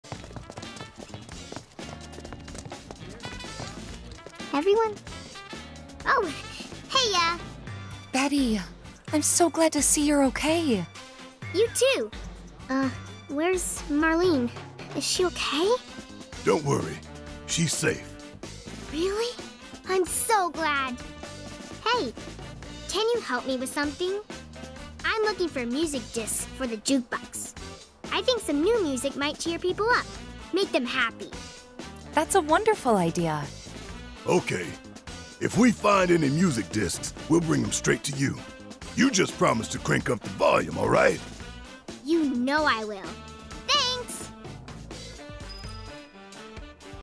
Betty wants to jam!
Betty thinks some tunes will cheer people up.